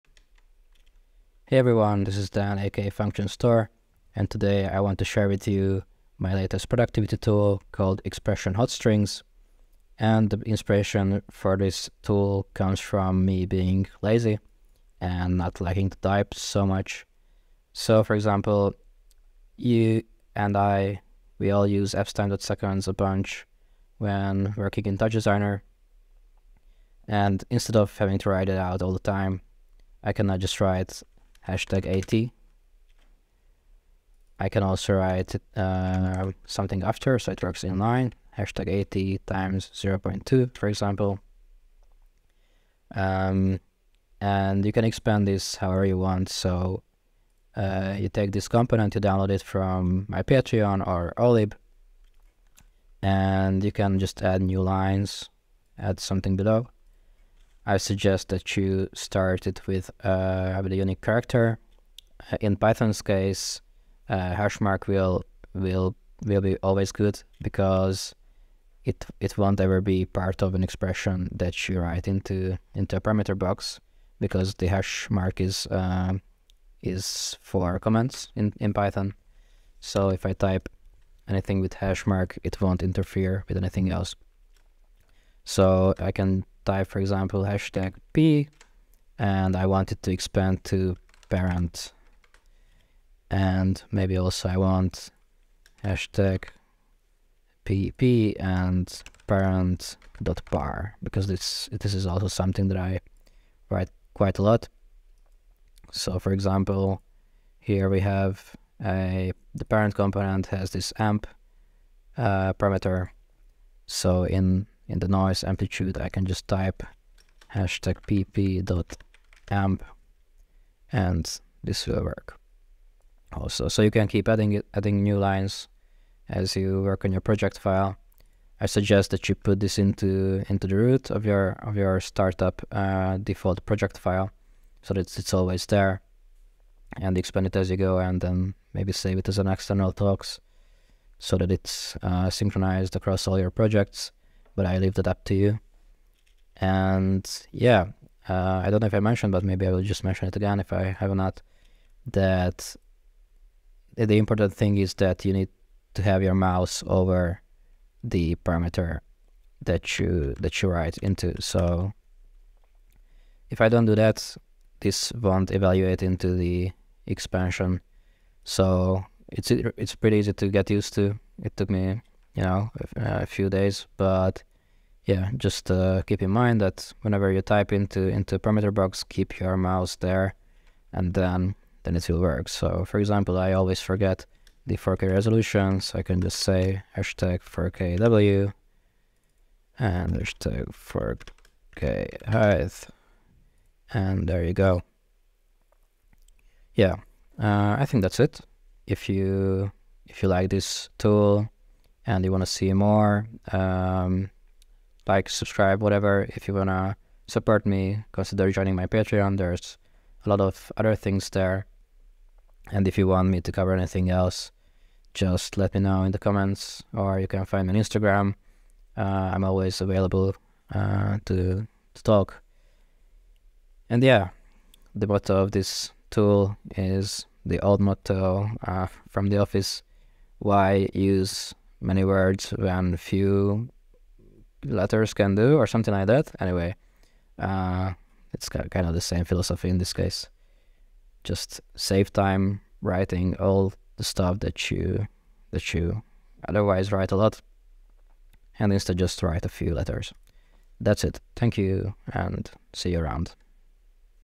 Save time writing expressions in TouchDesigner with this free tool
UPDATE: the content of this component now automatically sync between projects!

Do you type absTime.seconds a bunch? So do I, and many other things. But I am lazy too, that’s why I wrote a some lines of Python code in the form of this component to save you time typing!

In one column of this component's table you can define abbreviations, which when typed into a parameter expression box will expand to the text you define in the right column! Works inline too, for example if your abbreviation pp → expands to parent.par you can write pp.Customparam which will expand to parent.par.Customparam

Usage:
 - put this component anywhere in your project, I suggest placing it in root and adding it to your default startup project
- add a new line to the table
- first column is the abbreviation, suggest starting it with a unique character (like #)
- the second column is what it expands to when typed into a parameter expression
- when typing the abbreviation into the expression, finish it with hitting enter
- IMPORTANT: it only expands if you have your mouse over the expression box!!!
- The contents of the table sync between your projects! If you don't want this you can turn it off in the params